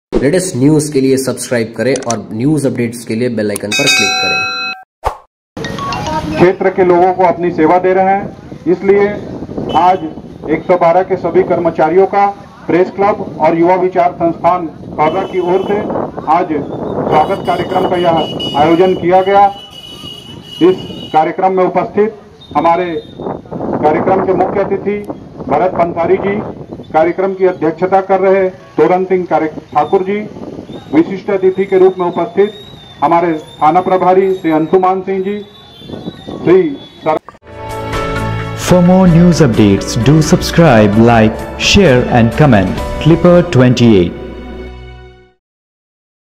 Chhattisgarh Kharora News: पुलिस पेट्रोलिंग के जांबाज जवानों एवं वाहन चालकों को किया गया सम्मानित
खरोरा: उत्कृष्ट कार्यो के लिए छत्तीसगढ़ शासन द्वारा चलाई जा रही पुलिस पेट्रोलिंग 112 के  पुलिस के जांबाज जवानों एवम वाहन चालकों का  छत्तीसगढ़ श्रमजीवी पत्रकार संघ  व युवा विचार संस्थान खरोरा द्वारा  श्रमजीवी पत्रकार संघ  प्रदेश अध्यक्ष अरविंद अवस्थी जी के मार्गदर्शन  से प्रतीक चिन्ह व श्रीफल देकर किया गया  सम्मानित खरोरा क्षेत्र में पुलिस पेट्रोलिंग 112 में तैनात पुलिस के सिपाहियों  व चालकों द्वारा क्षेत्र में हर विपदा में सड़क दुर्घटना गंभीर रूप से घायल व्यक्तियों गंभीर रूप से बीमार  व्यक्तियों को तत्परता के साथ अपना सहयोग प्रदान  किया जाता है.
 इस कार्यक्रम के मुख्य अतिथि भरत पसारी वार्ड क्रमांक 8 पार्षद अध्यक्षता तोरण सिंह ठाकुर  अध्यक्ष  युवा विचार संस्थान व विशिष्ट अतिथि अंशुमान सिंह थाना प्रभारी खरोरा थे  उनके द्वारा  पुलिस पेट्रोलिंग  112 के साथियों   जांबाज सिपाही भास्कर गणेश्वर डेहरिया जालम सिंह मनीष पटेल व वाहन चालक    मनोज चेलक हेमंत साहू डिगेशवर साहू प्रकाश चौहान  भूषण साहू  सुखनंदन साहू को  प्रतीक चिन्ह  व श्रीफल से उनके उत्कृष्ट कार्यो के लिए सम्मानित किया गया
 इस अवसर पर मुख्य अतिथि भरत पसारी आजादी  की 73वर्षगांठ की बधाई देते हुए कहा कि  ए के नंबर सबो बर  निश्चित तौर पर शासन की योजना से समाज के अंतिम व्यक्ति तक पुलिस की सहायता तत्काल मिल जा रही है
  कार्यक्रम के  अध्यक्षता  कर रहे तोरण ठाकुर   ने कहा हमारे पुलिस पेट्रोलिंग वाहन  112 के जवानों द्वारा  हर परिस्थितियों में अपने कर्तव्यों का निर्वहन करते हैं वह सभी की सहायता करते हैं
 थाना प्रभारी अंशुमन सिंह ने  उपस्थित सभी  लोगों का धन्यवाद ज्ञापित किया और कहां यह एक अच्छा प्रयास है
 इस अवसर पर  जोगिंदर सिंह सलूजा अध्यक्ष चेंबर ऑफ कॉमर्स  रमेश अमलानी  विकास सिंह ठाकुर कपिल नशीने पार्षद गण व  संदीप सोनी पत्रकार भी उपस्थित
थे
  श्रमजीवी पत्रकार संघ की ओर से  पत्रकार निलेश गोयल  ने सभी उपस्थित जनों का का  आभार व्यक्त  किया